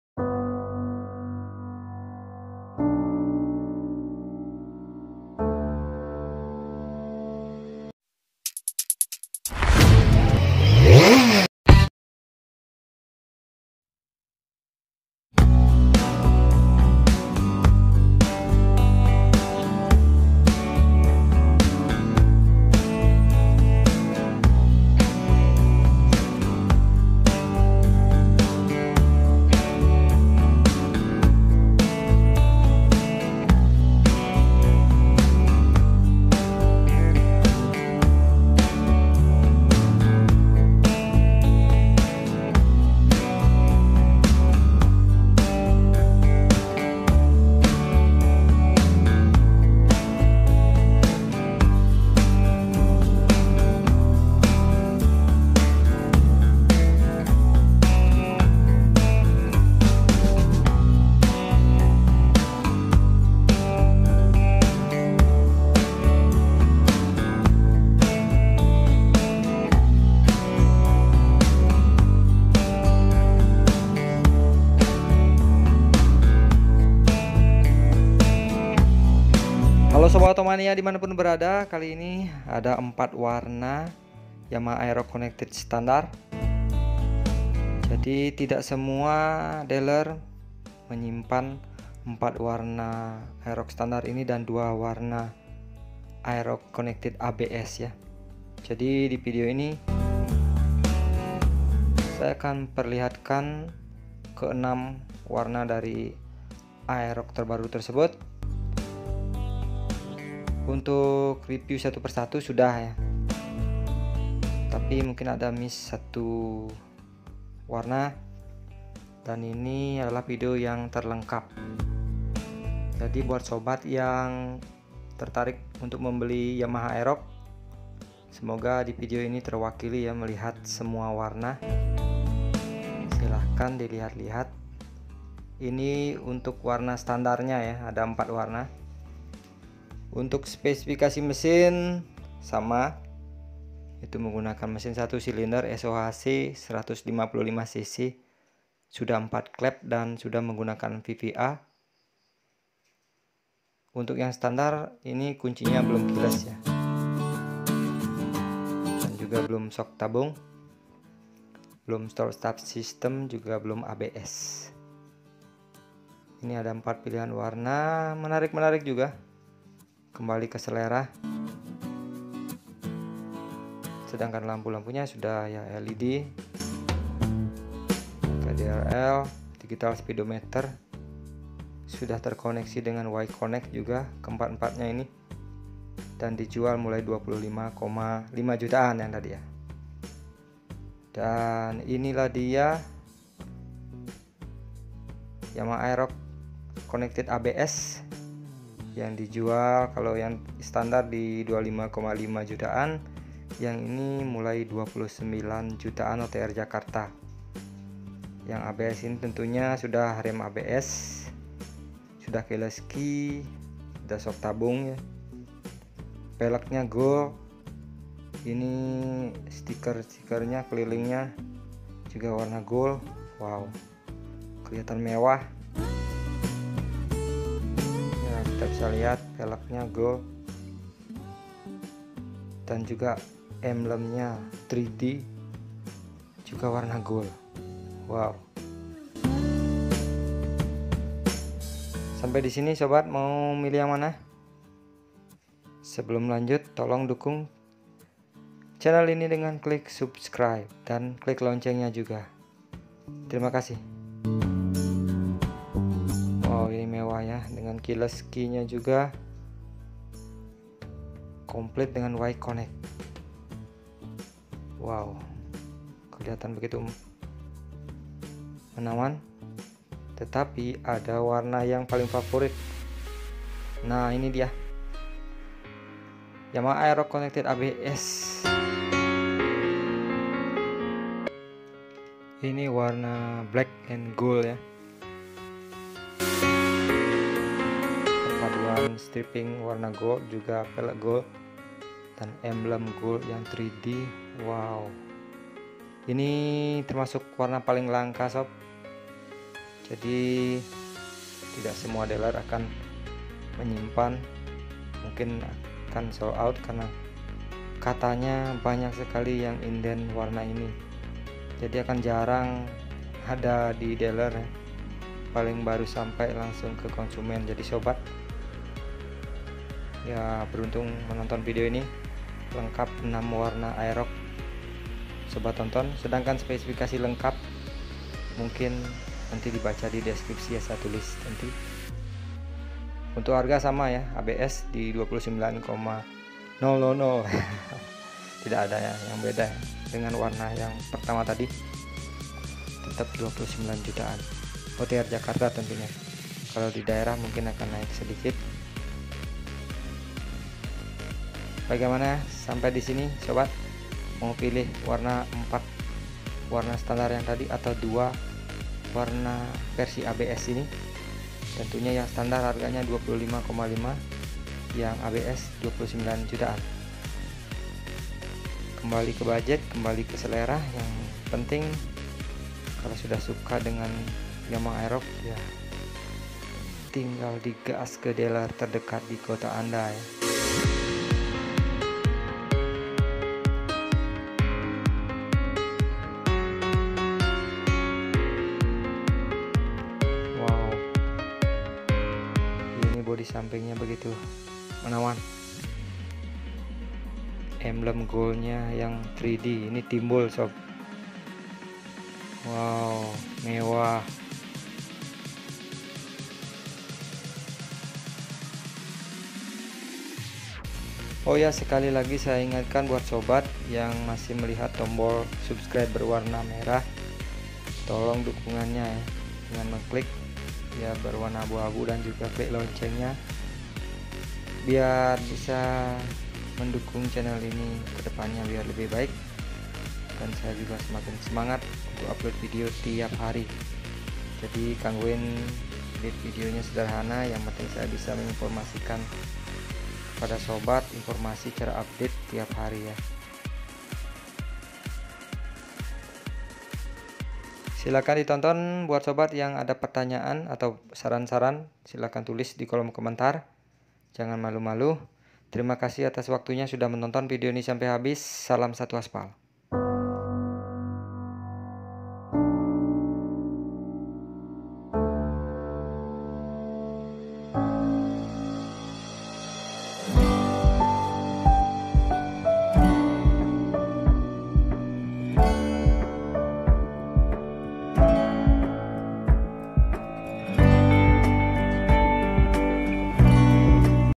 Pilihan warna komplit YAMAHA AEROX CONNECTED - 4 warna connected dan 2 warna connected abs
Divideo ini adalah semua pilihan warna Yamaha Aerox dari yang connected sampai connected abs. Yang aerox connected dijual mulai 25,5 jutaan dan yang aerox connected abs dijual mulai 29 jutaan. Semua nemiliki spesifikasi mesin 1 silinder sohc 155 cc ,vva,4 klep.
Fitur digital speedometer,LED,Y connect,smg,dan untuk type aerox abs dapat tambahan fitur abs,keyless key,emblem gold juga velg gold.